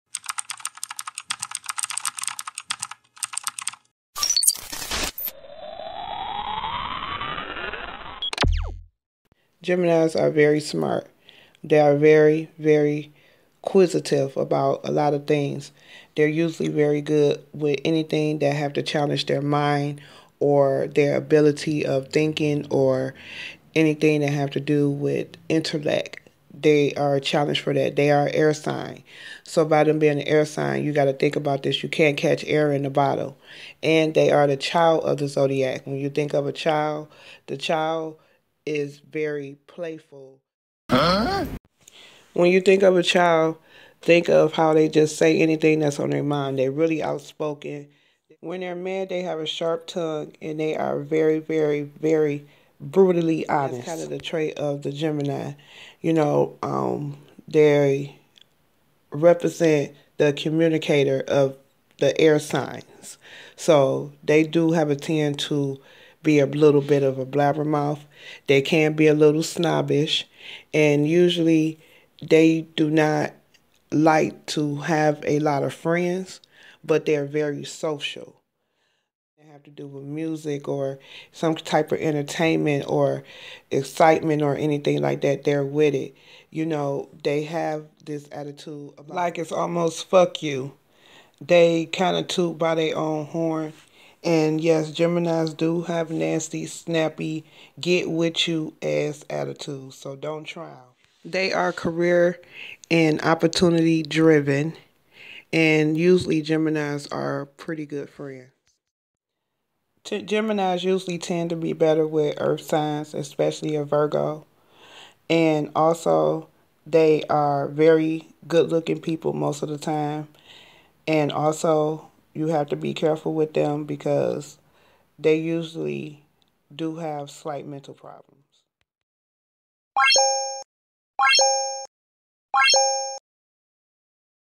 Gemini: "Child" of the Zodiac....
Patti Labelle, Mark Wahlberg, Laverne Cox, Amy Schumer, Marilyn Monroe, Andre 3000, Lauryn Hill, Kendrick Lamar, Heidi Klum just to name a couple more Geminis...

Geminis is known for there outspoken & free spirit nature.

Geminis are very In tune with there spiritual side, so they tend to be a leader of some sort with youth or elderly.(ministry)

Geminis love there family unconditionally with no strings.

Geminis can be cold hearted and will cut you off quick.

Geminis do have Tempers and Slight Attitude Problems!

Geminis will cheat in there relationships
if there not the side piece...winkwink

Geminis are freaks Baby!

Geminis either you like them or you don't simple...

Geminis usually are comfortable enough to try different sexual activities
( bi-sexual)

Geminis usually get along better in relationships with a Leo Virgo and Pisces and Libra.

Creator: Ms.Dee
Video/Sound Clips: VideoGuru
woodgeek store
ew.com
Astrology.com
Pinterest.com